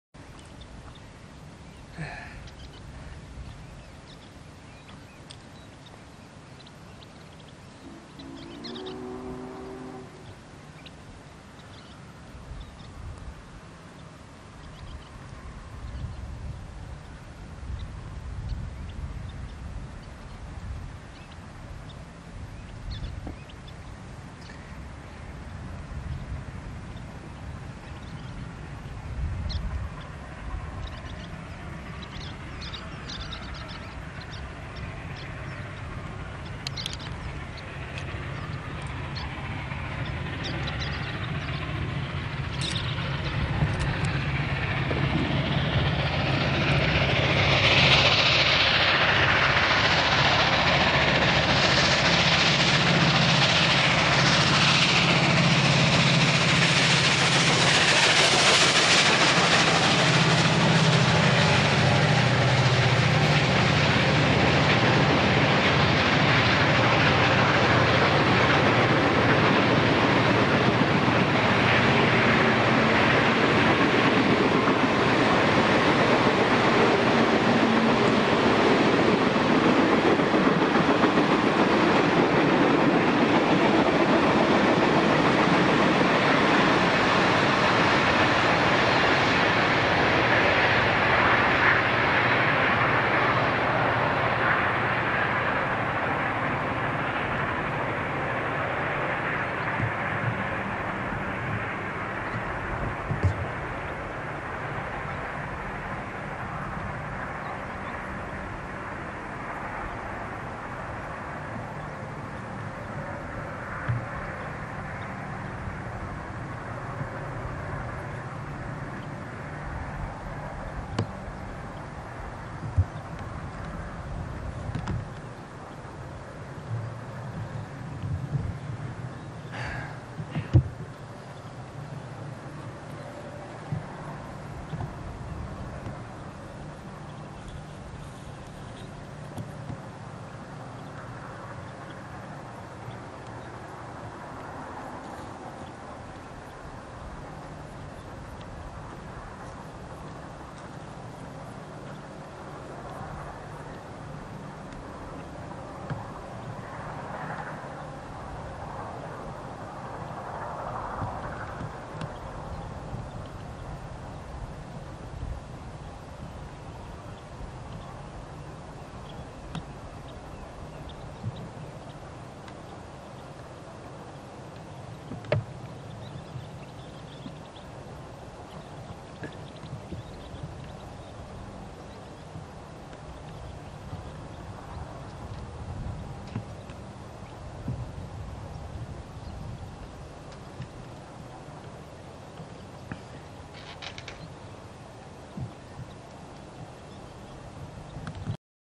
Steam Railroad Series 147 - UP 844+6936 Eastbound 7-4-1990 Full Throttle King Hill,Idaho
Hope You Have A Blast watching this one! Along with UP 844, UP 6936 Quickly became another one of my favorite operating Locomotive Icons, Spurred on by reading Ted Benson's book titled "Echoes Down The Canyon". The 6936 is a natural to be preserved as part of UP's "Heritage Fleet"